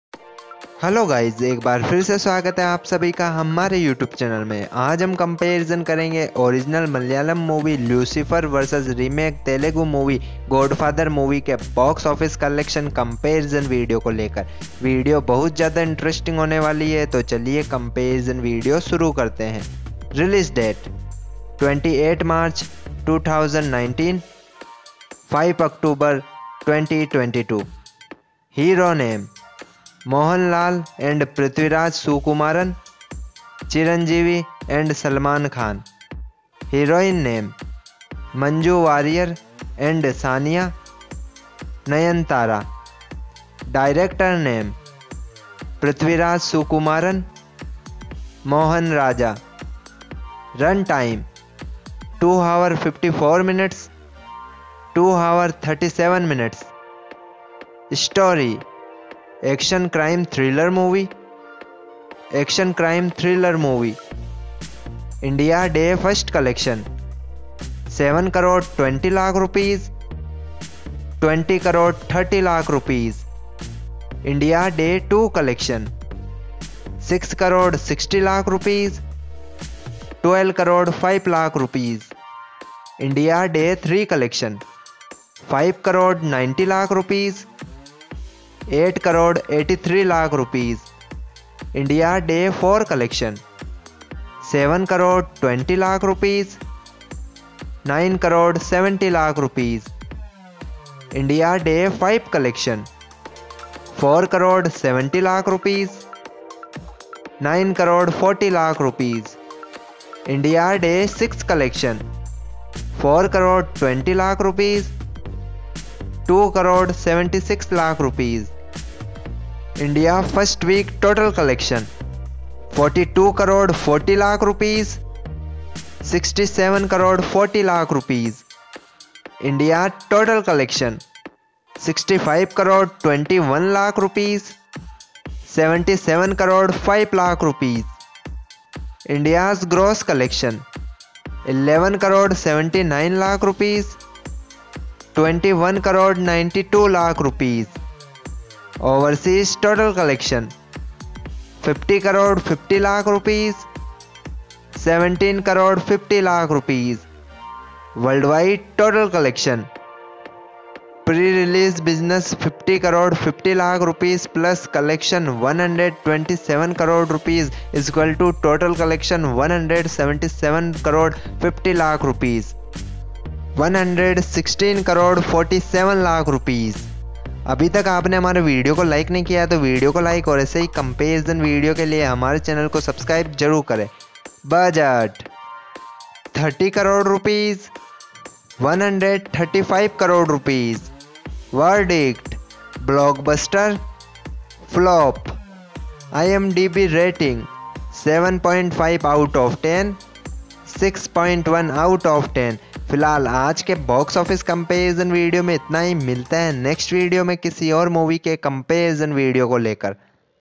Original Lucifer vs Remake Godfather।mohan lal vs cheeranjivi
Original Lucifer vs Remake Godfather movie box office collection comparison।mohan lal vs cheeranjivi